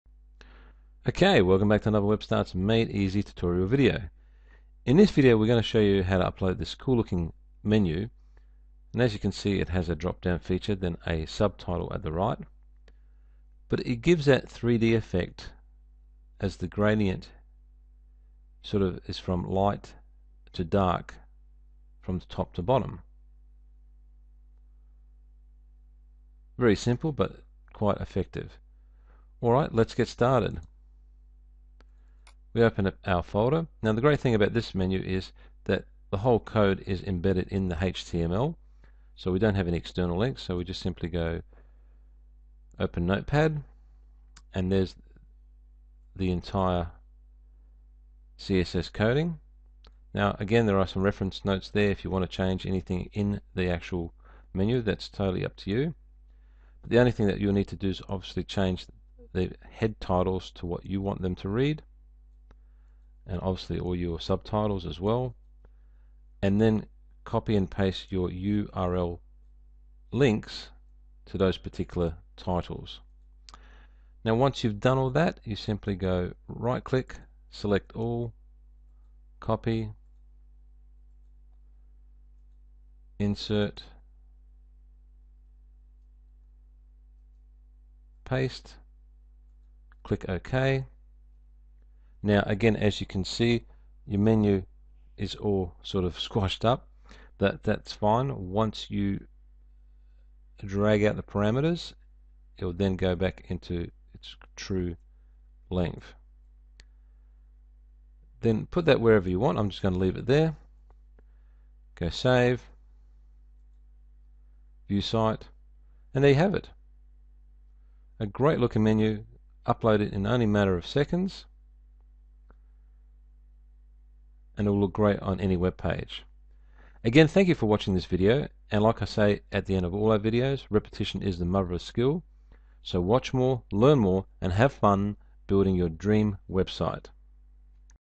How to upload 3D Gradient Dropdown CSS Menu into WebStarts
In this video we show you how to upload 3D Gradient  Dropdown CSS Menu into WebStarts